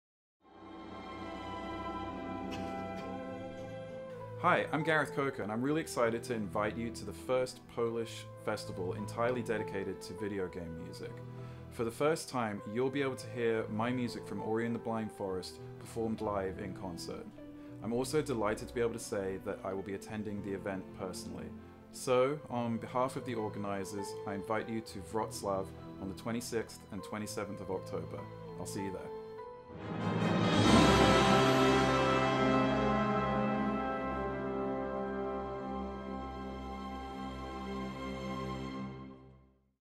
Gareth Coker zaprasza Was na Game Music Festival!
👂Posłuchajcie, co Gareth Coker ma Wam do przekazania!